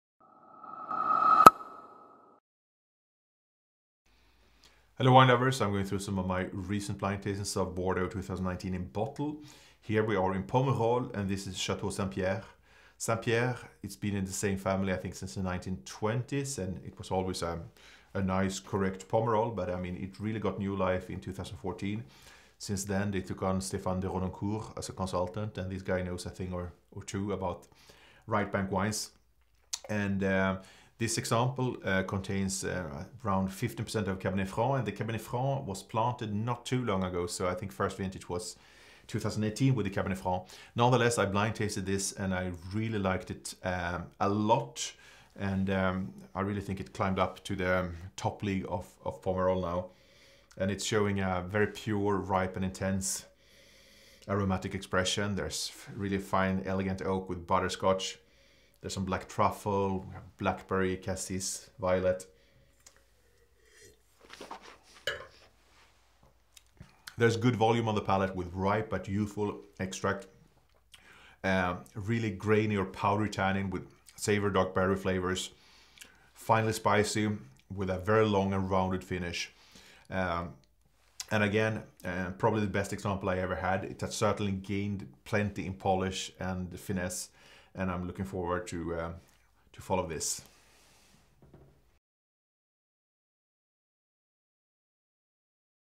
Château Saint Pierre - 2019 - Pomerol
Inky dark youthful colour with a flush of purple. Pure, ripe and intense nose offering fine oak and butterscotch, some truffle with blackberry; cassis and violet notes. It shows good volume on the palate with ripe but youthful extract and tannin, savoury dark berry flavours with a fine spiciness and long rounded finish; it has certainly gained in polish and finesse.